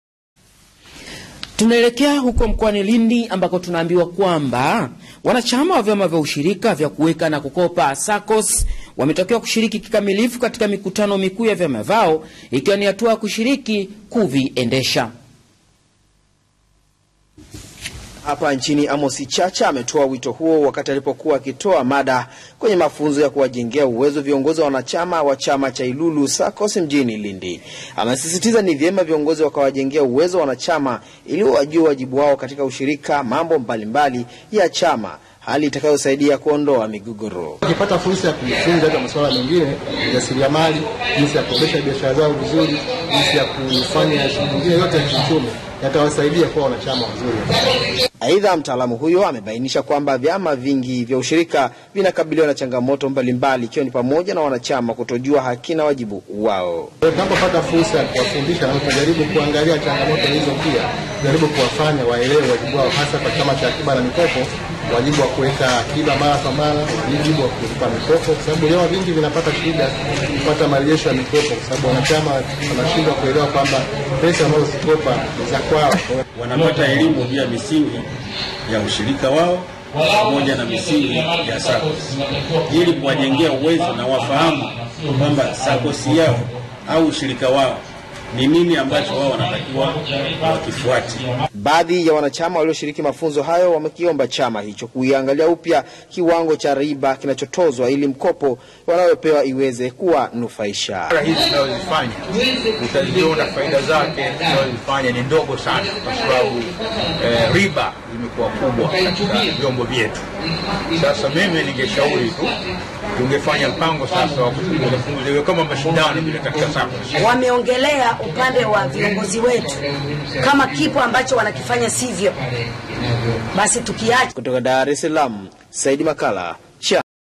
Wito Kwa Wanachama Vyama Vya Kukopa Fedha
Wanachama wa vyama vya kukopa wametakiwa kushiriki mara kwa mara katika vikao vya kitaifa